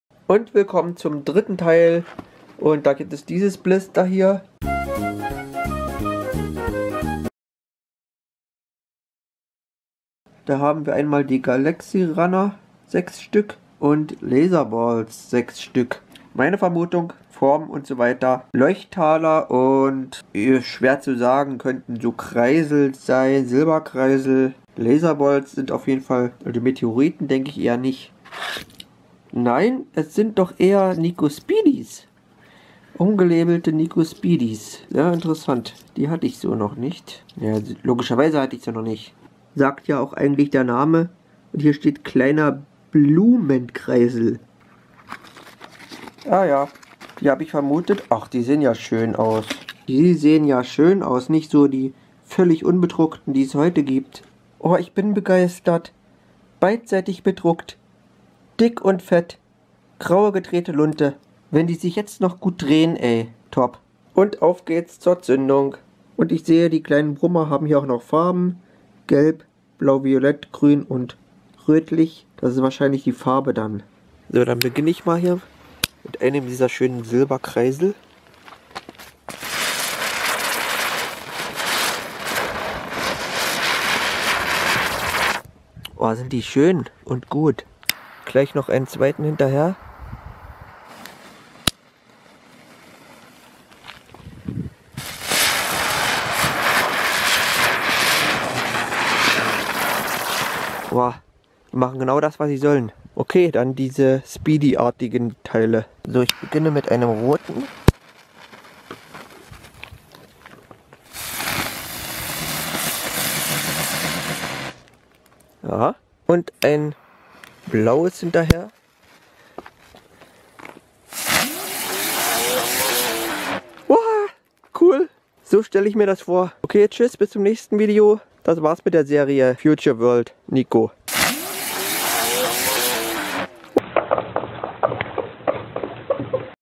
Nico Future World Blister 3
So, jetzt das dritte Blister aus meiner Sammlung. Ich hatte die doppelt, deshalb habe ich eins jeweils auf gemacht.